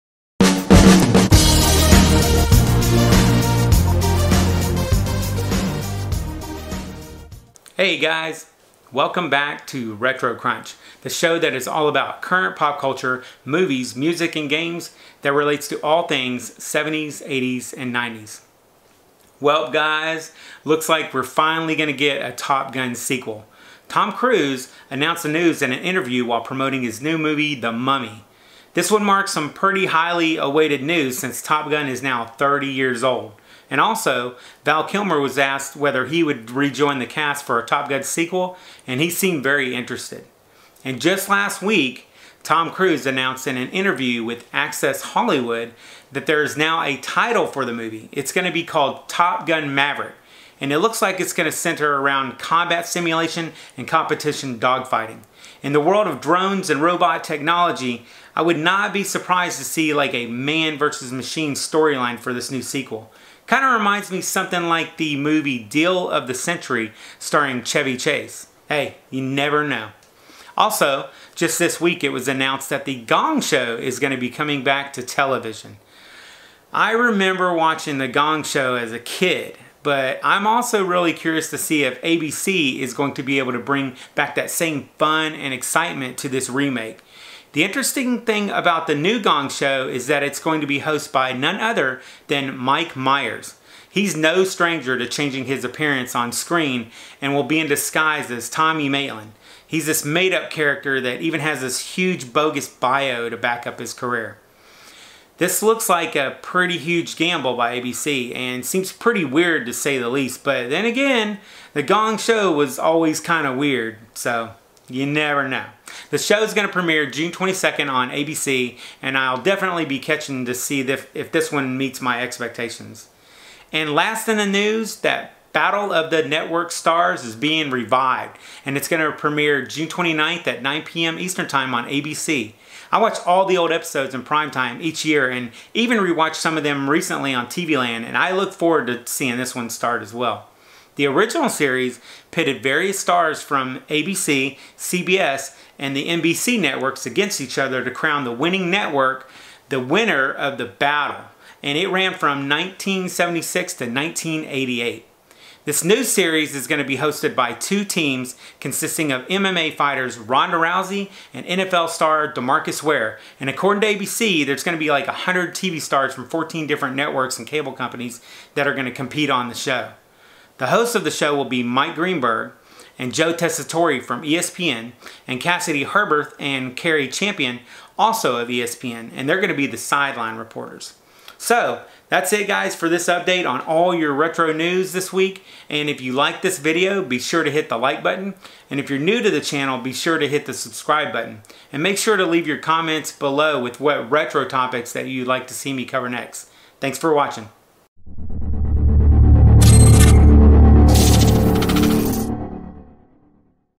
Top Gun Maverick!! | GONG SHOW Returns | Battle Of The NETWORK STARS Is BACK!
The GONG SHOW Returns!! And ABC's Battle of the NETWORK STARS is back!  Starring Ronda Rousey and Demarcus Ware as coaches.  Also, TOP GUN MAVERICK news and Retro and Nostalgia fun in this episode!

Hey guys, Welcome back to RETRO CRUNCH, the show that is all about Current Pop Culture, Movies, Music, and Games that relates to all things 70s, 80s, and 90s!

Welp guys!  Looks like there's going to be a Top Gun sequel.  Tom Cruise announced the news in an interview on an Australian morning show while promoting his new movie Mummy.  This would mark some highly awaited news since Top Gun is now 30 years old.  Also, Val Kilmer was asked about whether he would rejoin the cast for a Top Gun sequel and he was very interested - so all we need is for Goose to be resurrected and were all set!  Kinda makes you wonder if a Nerds reboot should be in the works???  Hmmm…

And just last week Tom Cruise announced in an interview with Access hollywoo that  theres now a title:  Top Gun: Maverick and it looks like its going to center around combat simulation and competition dogfighting.  In the world of drones and robot technology, I would not be surprised to see a man vs machine storyline for this new sequel.  Maybe something like the movie Deal of the Century, staring Chevy Chase.  Never know?

Also, Just this week, it was announced that The Gong Show is going to be coming back to television.

I remember watching the GONG SHOW as a kid, but im really curious to see if ABC is going to be able to bring that same fun and excitement to this remake. The interesting thing about the new GONG SHOW is the host will be none other than Mike Myers - whos no stranger to changing his appearance on screen and will be in disguise as Tommy Maitland, a made up character that even has a huge bogus bio to backup his career.

This looks like a huge gamble by ABC, and seems pretty weird to say the least, but then again, the GONG SHOW was pretty weird anyways.   The show will premiere June 22nd on ABC, so I'll deffinately be catching this one to see if it lives up to expectations.

And last is news that Battle of the Network Stars is being revived and will premiere on June 29th at 9 PM Eastern time on ABC.  I watched all of the old episodes in primetime each year and even rewatched some of them recently on TVLAND and look forward to this one starting!

The original series pitted various stars from the ABC, CBS, and NBC networks against each other to crown the winning network the winner of the battle and it ran from 1976 to 1988.

This new series is going to be a hosted by 2 teams consisting of MMA fighter Ronda Rousey and NFL star Demarcus Ware and According to ABC, there will be 100 TV stars from 14 different network and cable companies that will compete on the show.

The hosts of the show will be Mike Greenberg and Joe Tessitore from ESPN and Cassidy Hubbarth and Cari Champion, also of ESPN will be the sideline reporters.

So that’s it guys for this update on all your RETRO news this week, so If you liked the video, be sure to hit the LIKE button and if your new to the channel, be sure to hit the subscribe button and leave your comments below with what RETRO topics you'd like to see me cover NEXT!